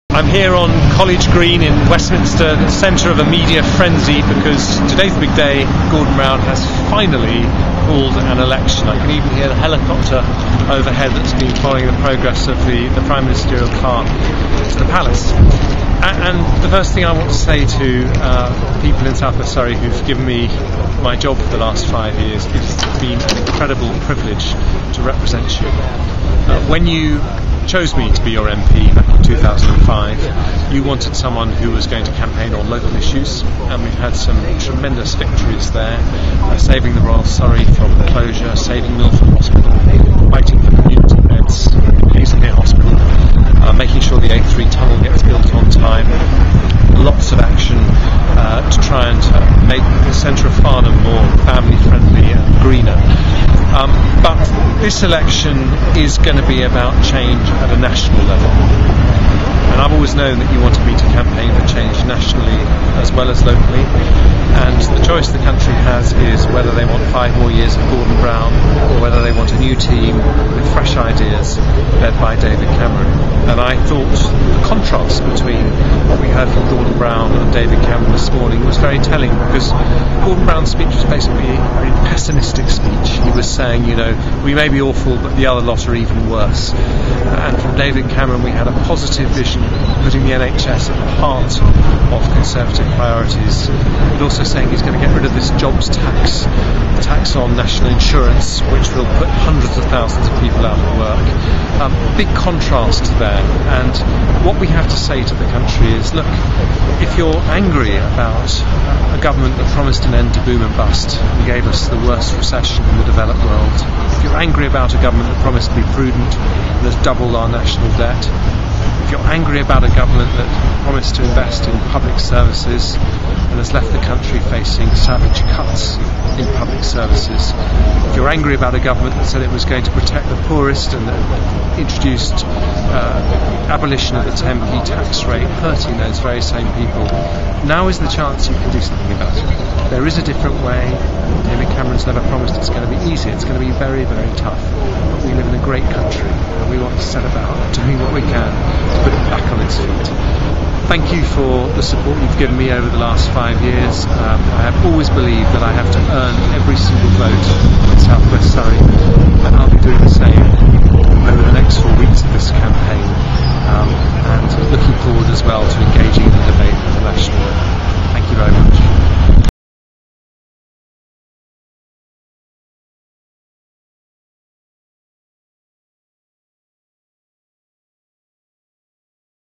Jeremy Hunt election campaign message
Jeremy Hunt heralds the start of the 2010 election campaign following Gordon Brown's announcement